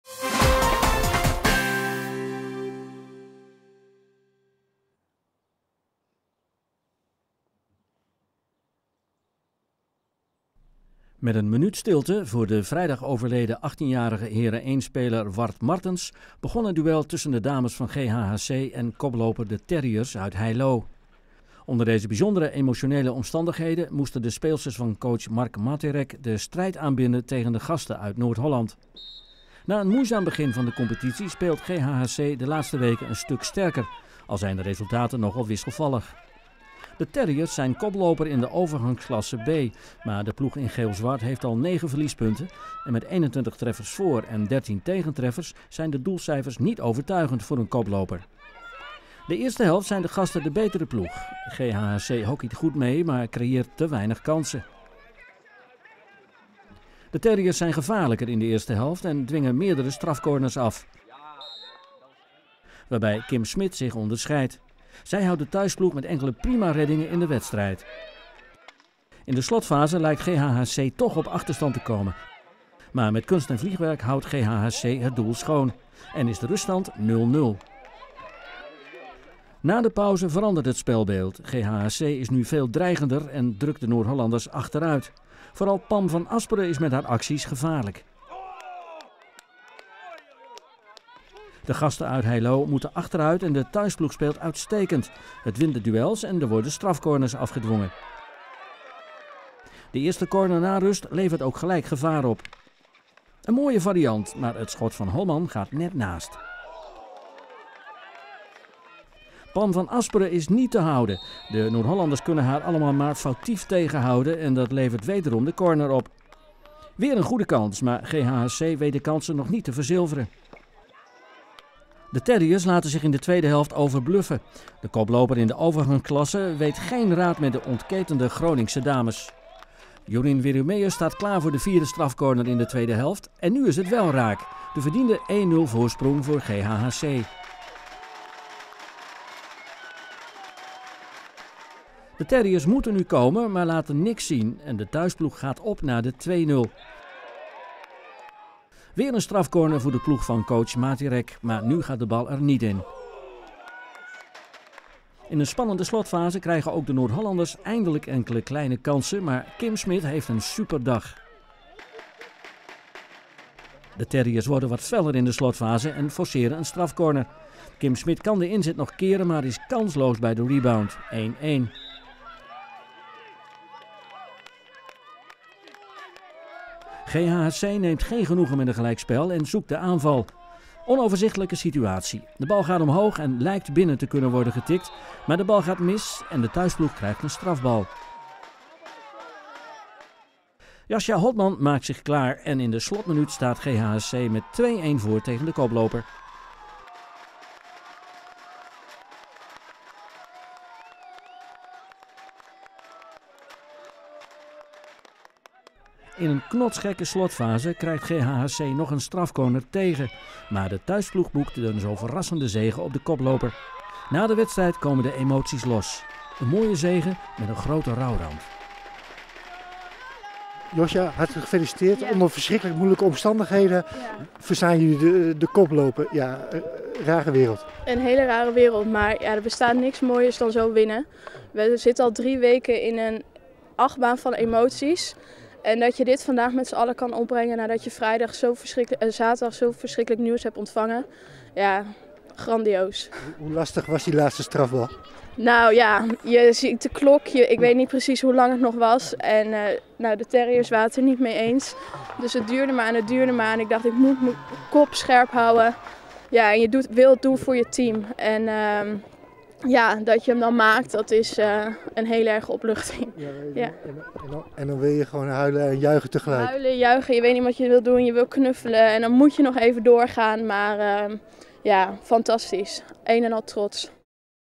Dames GHHC winnen emotioneel duel
De dames van GHHC hebben in de overgangsklasse B met 2-1 gewonnen van koploper De terriers uit Heiloo.

Het duel begon met een minuut stilte wegens het overlijden van  Jard Martens, speler van Heren 1.

In de eerste helft waren de gasten beter, maar dankzij een uitblinkende keeper bleef de ploeg van coach Marc Materek overeind.

Na rust was GHHC de betere ploeg en kwam verdiend op een 1-0 versprong,  maar de Noord Hollanders kwamen tien minuten voor tijd  op gelijke hoogte.

Maar dankzij een benutte strafbal in de laatste minuut door Jasja Holtman, behaalde de thuisploeg en verdiende zege.

GHHC maakte daarna, onder luidapplaus, met de tranen in de ogen, een ereronde

Coach Materek was vooral trots op zijn ploeg, die onder bijzondere zware omstandigheden, een prima wedstrijd speelde.